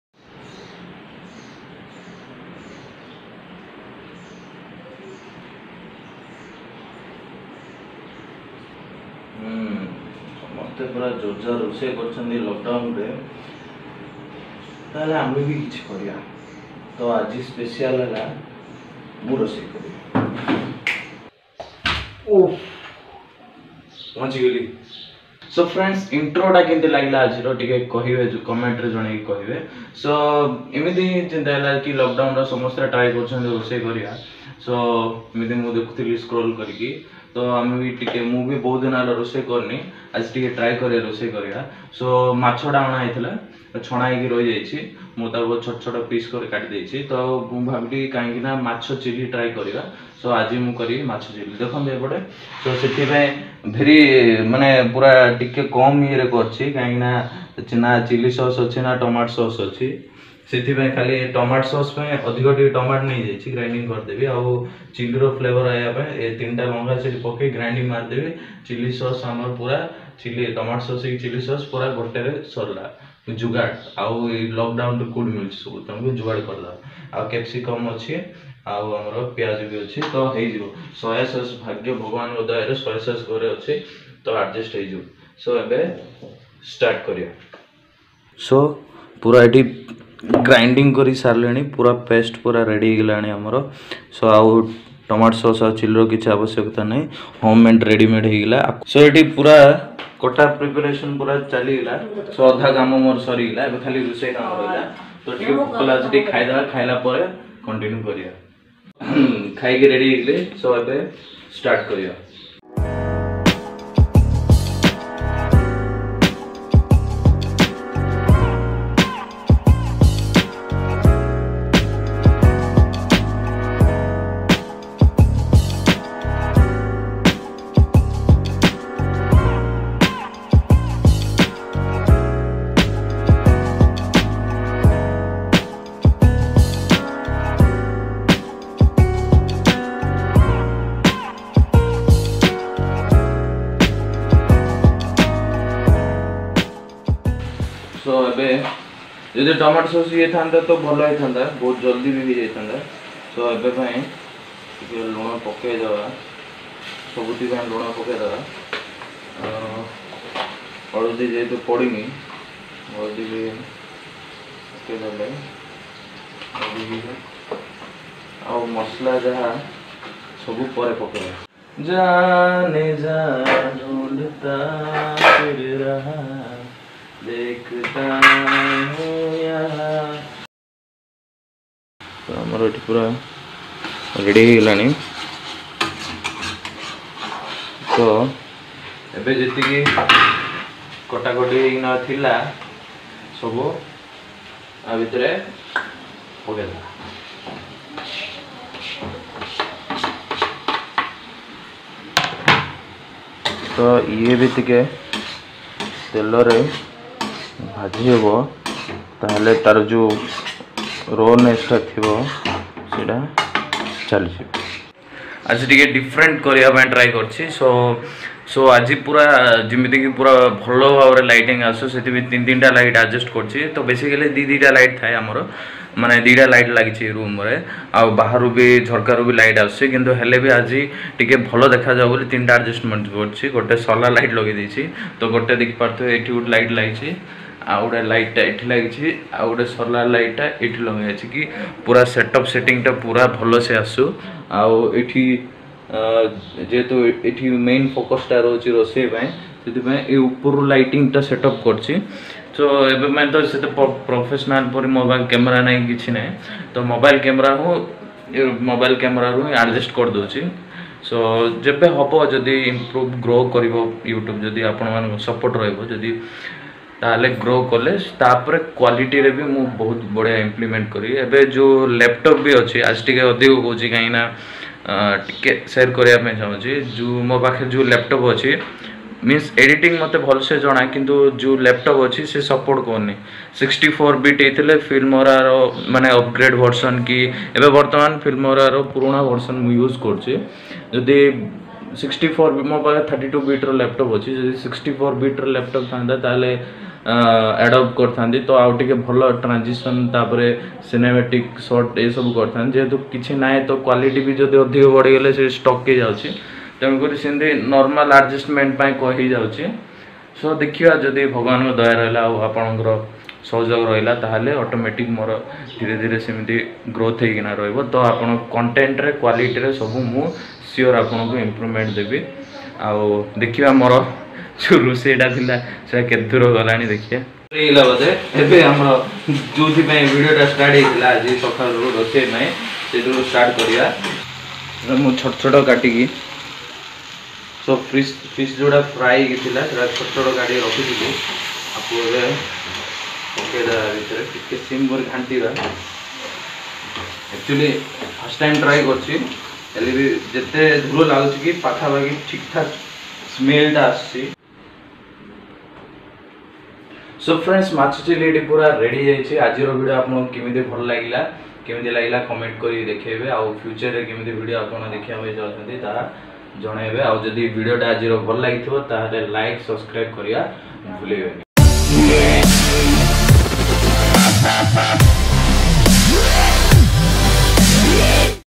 Cooking fever....| Odia vlog | Odia Boy Braja
After a long time i cook fish chilli hope you like this vlog.(Odia vlog video)

Odia vlog video #6